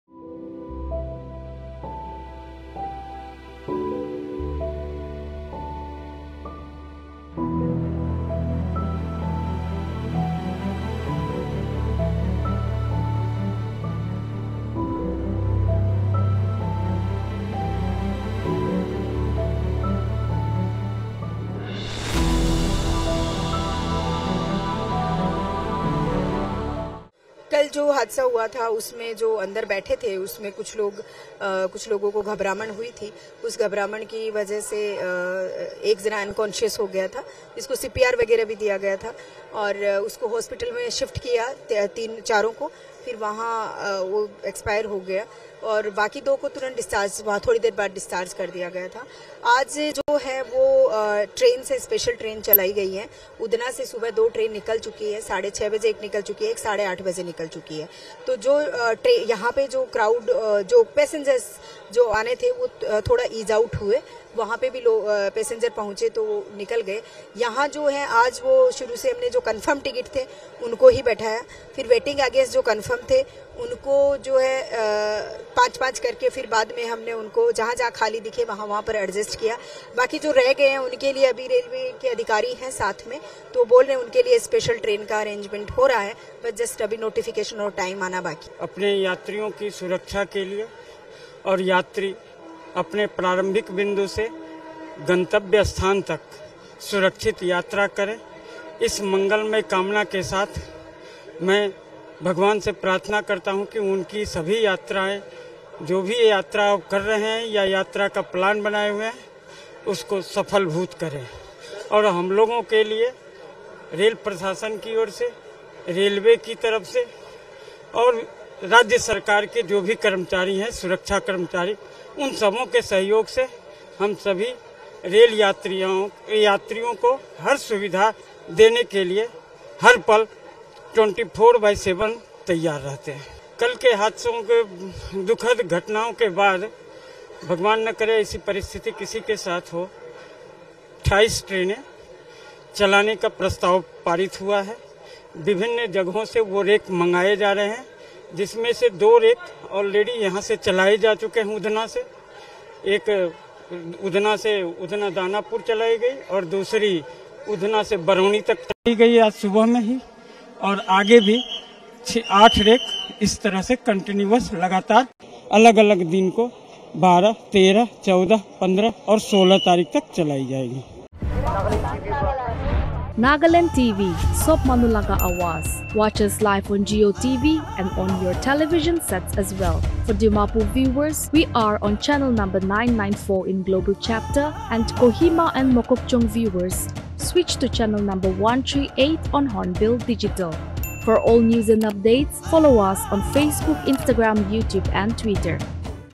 ONE DEAD, SEVERAL INJURED IN STAMPEDE AT GUJARAT'S SURAT RAILWAY STATION
On November 11, a stampede at the Surat railway station in Gujarat killed one person and injured several others. Reportedly, A huge crowd of people going to their native places for Diwali had gathered at the station to travel to different states and while boarding the train, a stampede occurred, killing one person, while several others fainted and were injured. Meanwhile, Surat's MP and state railways minister, Darshana Jardosh, visited the hospital to meet the injured